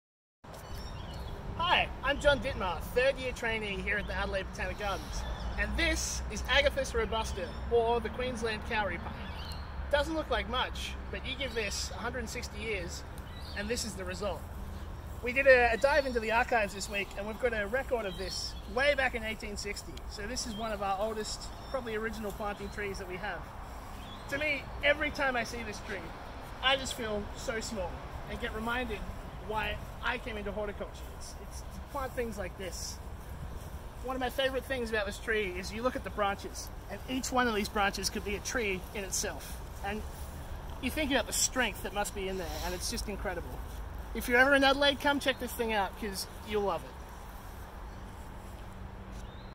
BGANZ plantchallenge with Botanic Gardens and State Herbarium Horticultural Trainee, Jon Dittmar
The Botanic Gardens Australia and New Zealand Inc (BGANZ) has challenged everyone to share their isolation plant love story or 'plantspiration'.

Here, Botanic Gardens and State Herbarium of South Australia Horticultural Trainee, Jon Dittmar, shares his passion for the 160-year old Agathis robusta (Queensland Kauri Pine) at Adelaide Botanic Garden.

From your favourite, weird and wonderful, to veggies, natives or family heirlooms… we now plantchallenge you to share your plant story in a 30-45 second video during the month of MAY. Be sure to tag BGANZ and use the hashtag on social media.

for more plantchallenge videos.